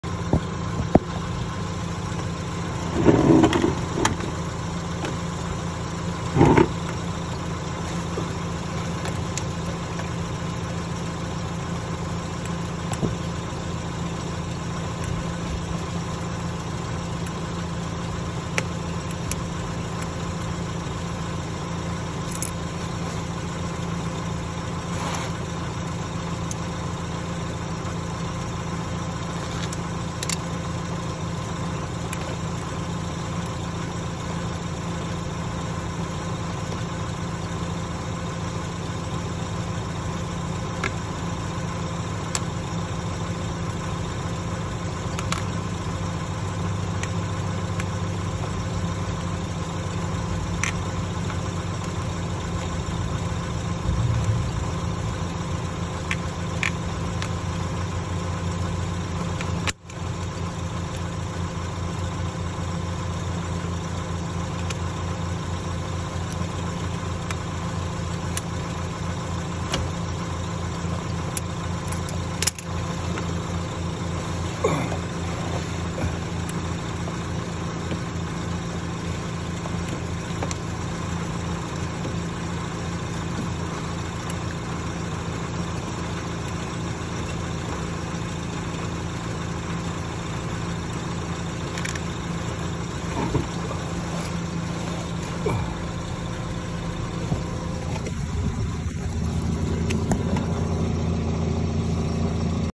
Car fixing water coolant drain mini cooper bmw petrol mechanic repair
Mini cooper r56 1.6 petrol 2010  how to drain water coolant